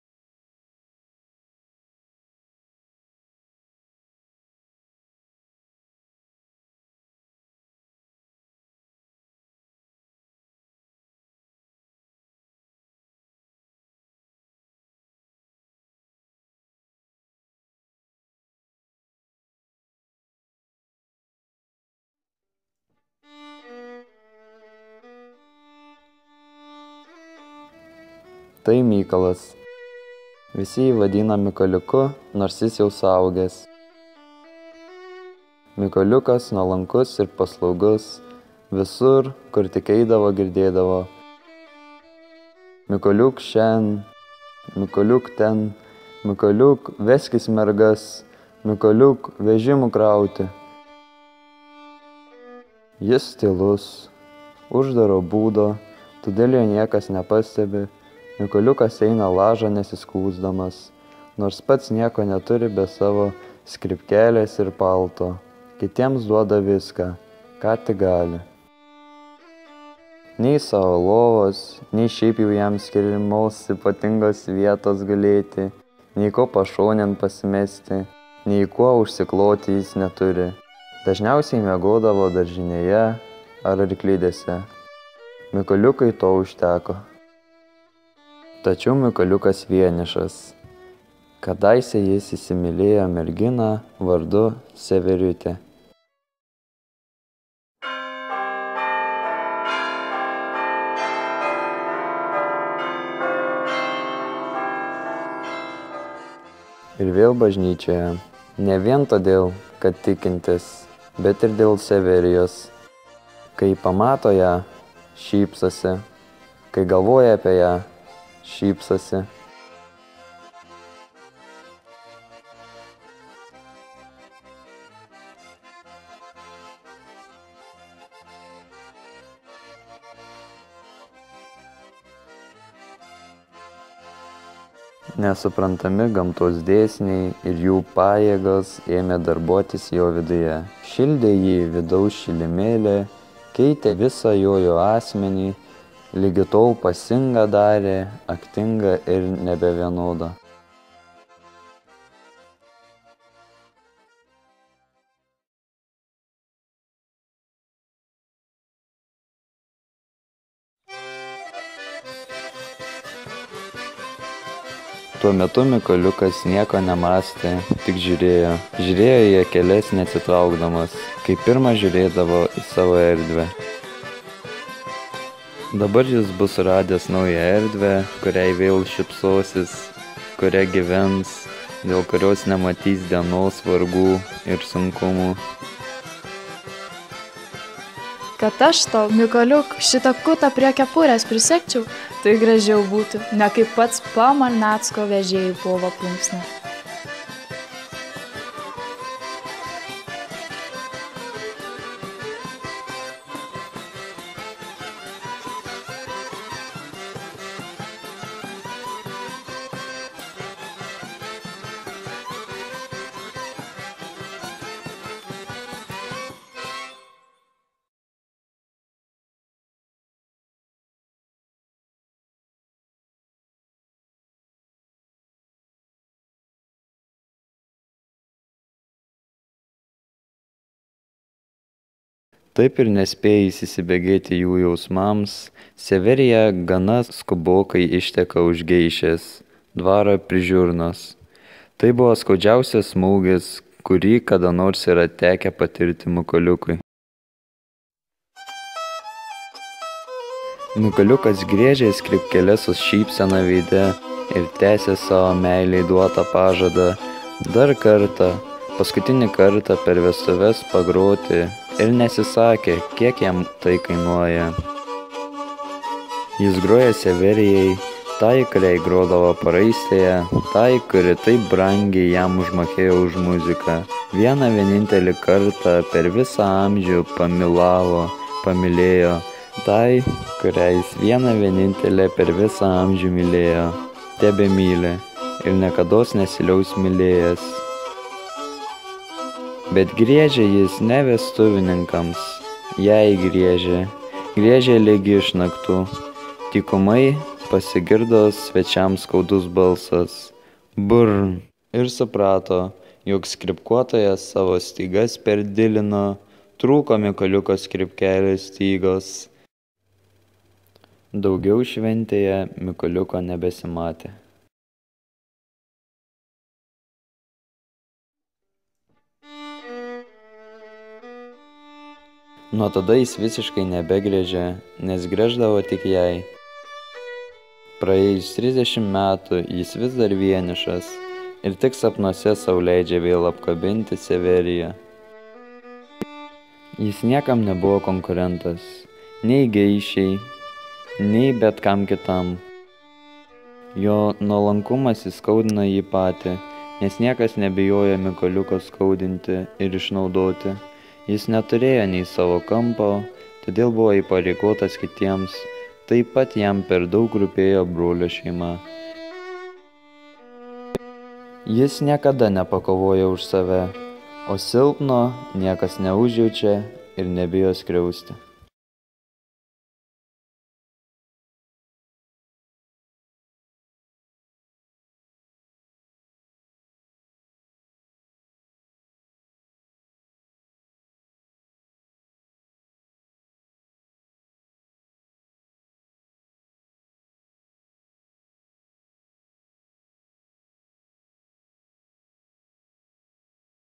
Juozas Tumas -Vaižgantas „Dėdės ir dėdienės". Kodėl Mykoliukas lieka vienas?
Juozo Tumo - Vaižganto 150-osios gimimo metinėms paminėti.